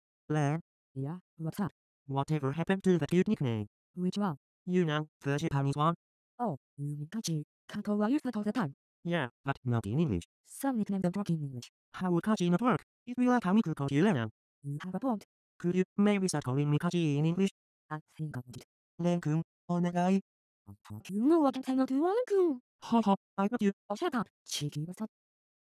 [TALKLOID] Nicknames
Len and Kaito have a discussion about nicknames.

Yes I used ProSeka chibis in a talkloid. They need to be used more often bc they're adorable.

VOICEBANKS USED:
KAITO V3 English and Soft
Kagamine Len V4 English

Thank you to everyone who helped me! 5,000 is the next goal!